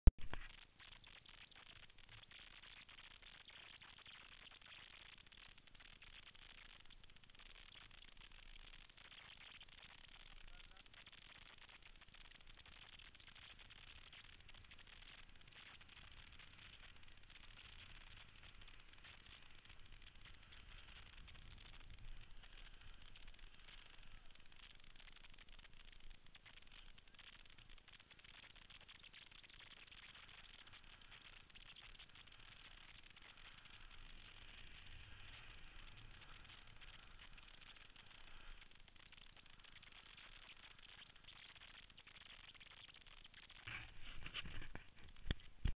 Áďa - den první (2011)
Točeno kamerkou GoPro připevněnou na staré žehličce :-) Malbun, březen 2011